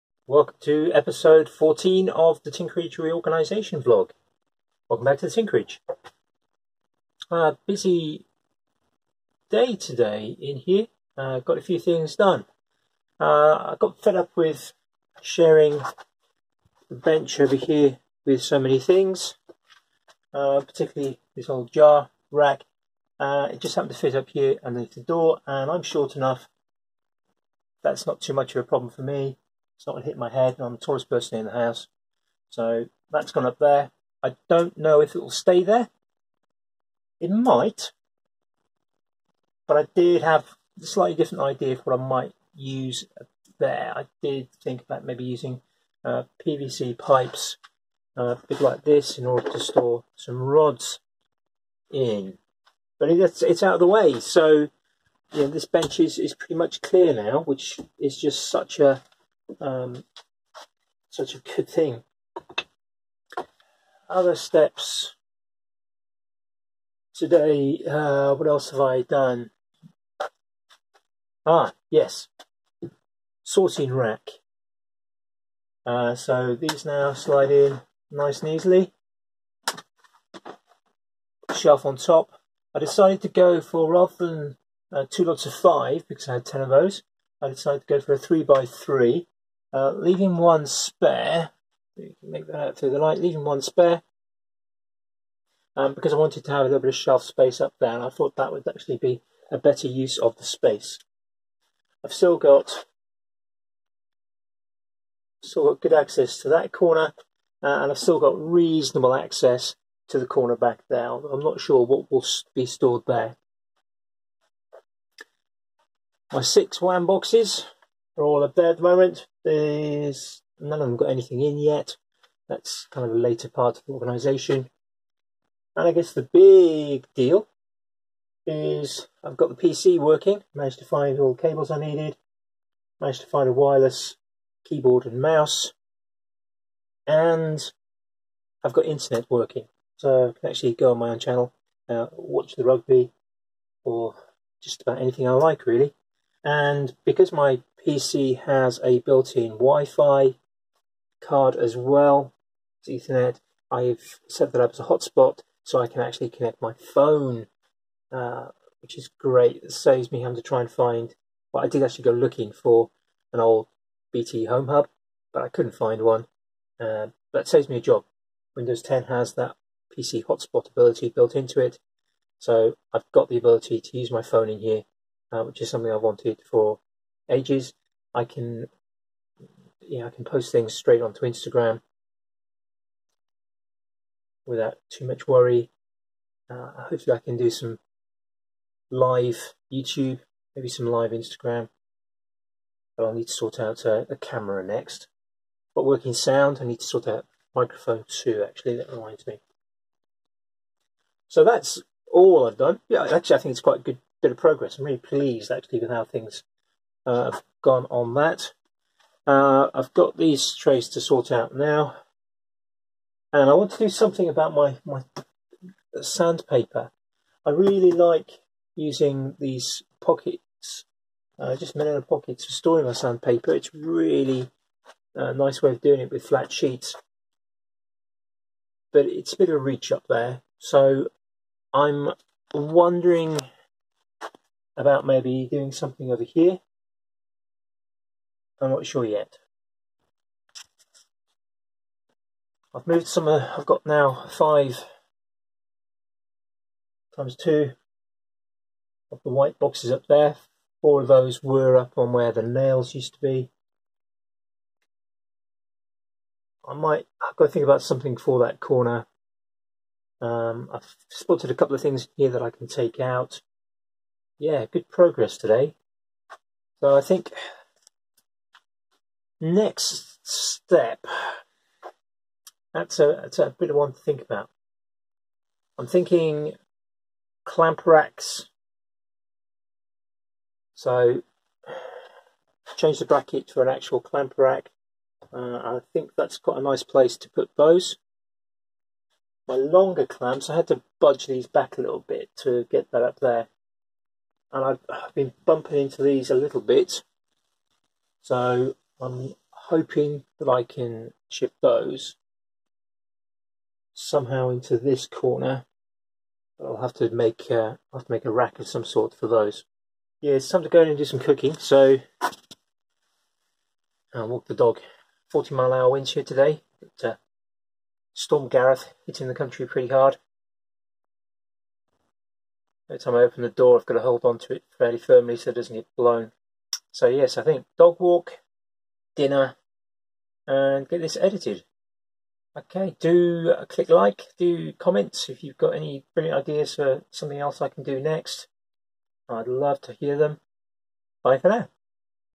TFTT035 Tinkerage Reorganisation VLOG 14
It's been a windy day in the UK so that means making good progress on the reorganisation of the Tinkerage.